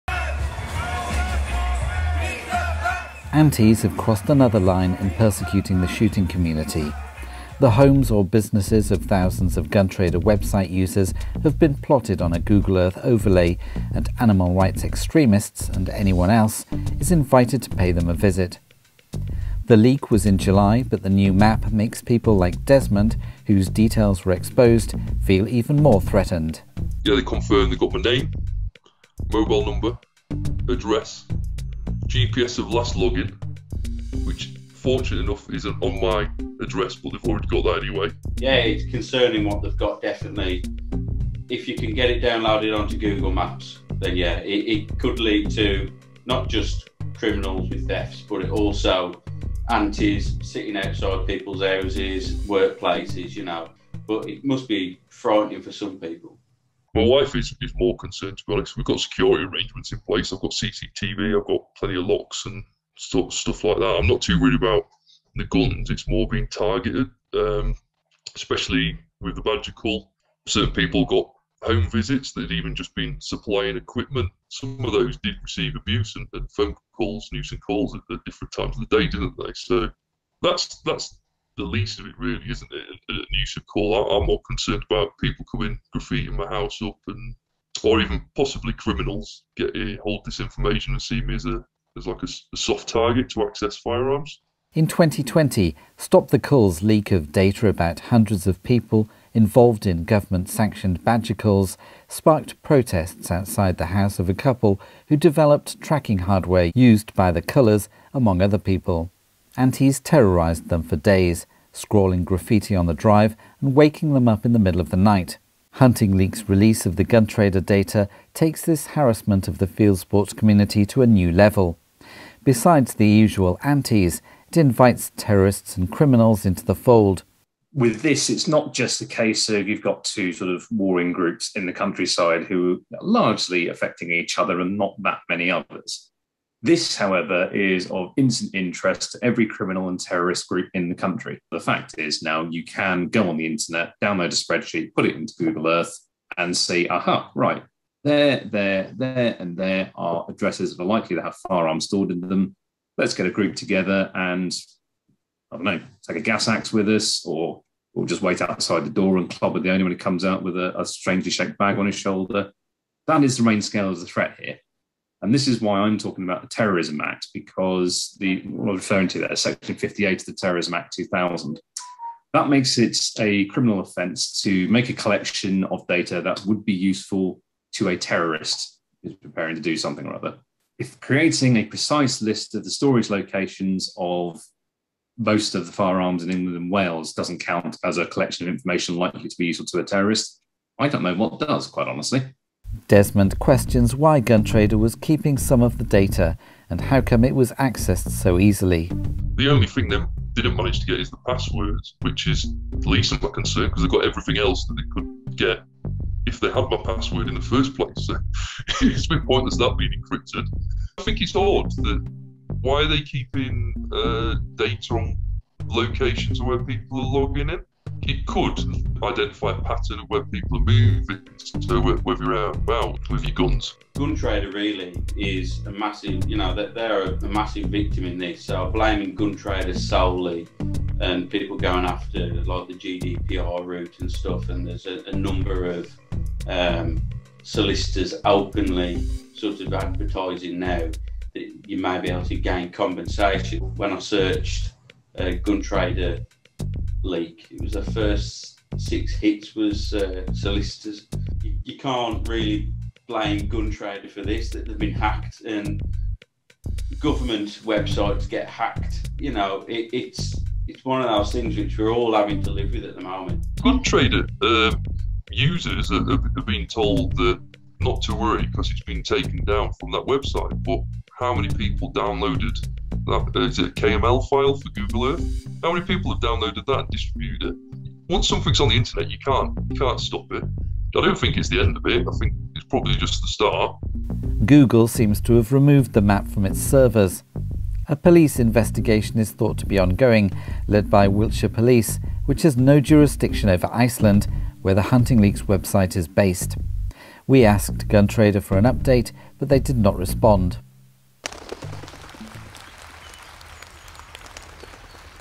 Hacked details of 100,000+ UK gun owners published online
After antis got their hands on Guntrader.uk website user data, many UK gun owners were nervous. That was aggravated by the release of a Google Earth map pinpointing the addresses of up to 111,000 users of the Guntrader.uk website. Have antis crossed a line from activism to terrorism? We talk to some of the people involved.